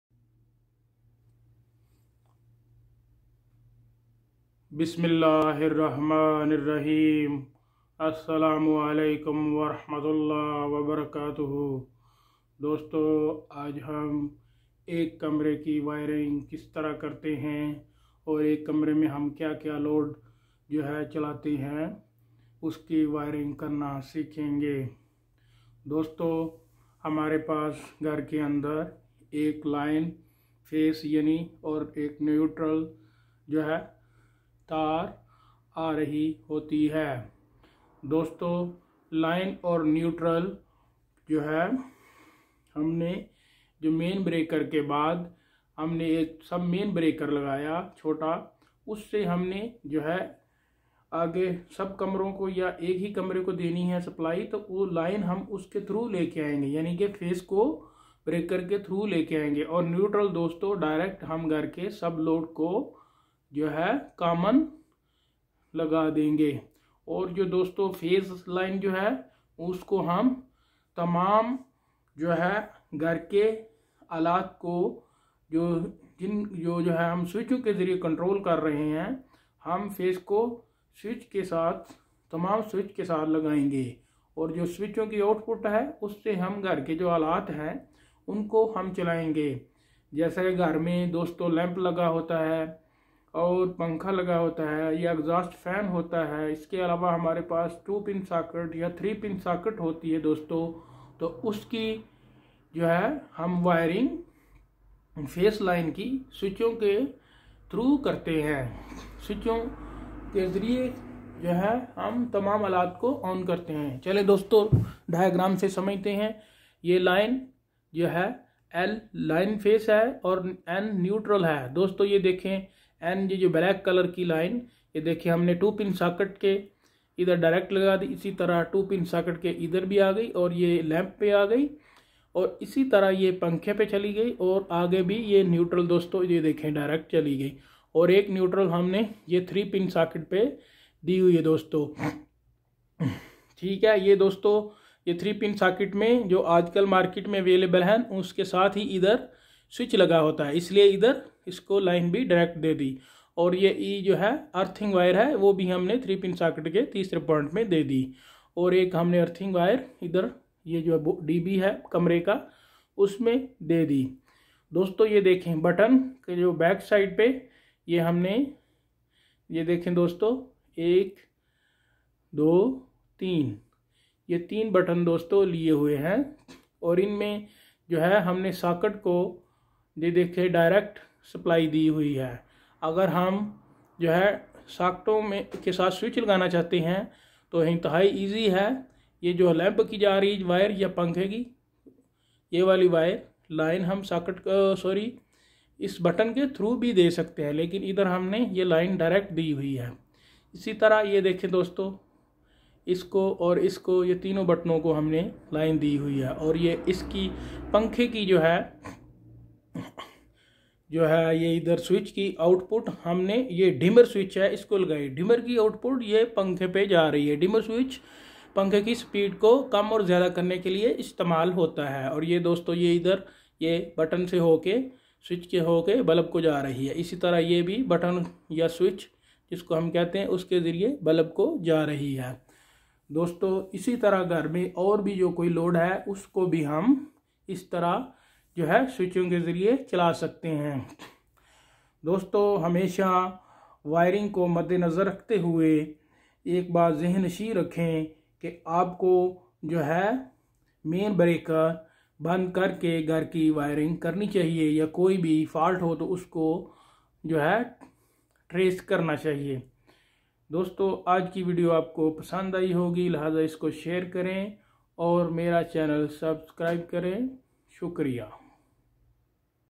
One Room Electrical Wiring Connections Diagram| بجلی کی وائرنگ ایک کمرے کی مکمل سیکھیں
اب انتہائی آسان طریقہ کے ذریعے ایک کمرے کی بجلی کی وائرنگ  مکمل کریں یا سیکھیں                                    Now complete or learn the electrical wiring of a room in a very simple way